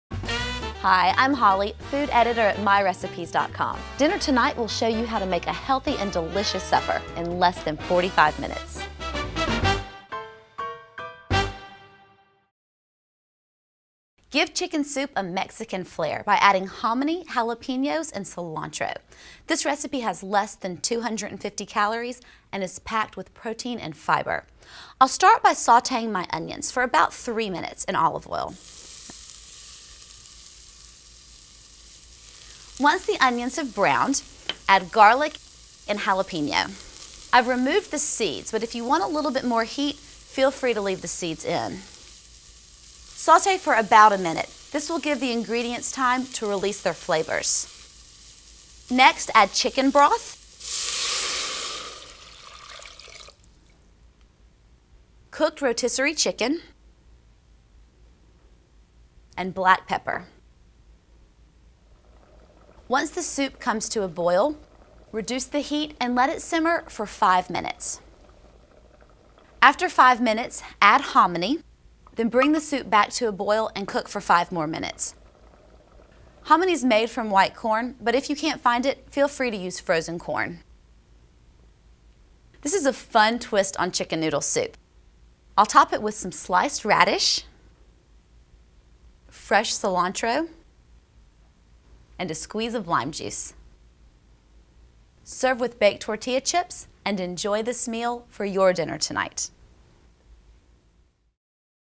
How to Make Mexican Chicken Hominy Soup | MyRecipes
Give chicken soup Mexican flair by adding hominy, jalapeño, and cilantro.

1 tablespoon olive oil
1 3/4 cups chopped onion
3 garlic cloves, minced
1 jalapeño pepper, seeded and minced
2 cups shredded skinless, boneless rotisserie chicken breast
1/4 teaspoon freshly ground black pepper
2 (14-ounce) cans fat-free, lower-sodium chicken broth
1 (15.5-ounce) can hominy, rinsed and drained
1/2 cup thinly sliced radishes
2 tablespoons fresh cilantro leaves
4 lime wedges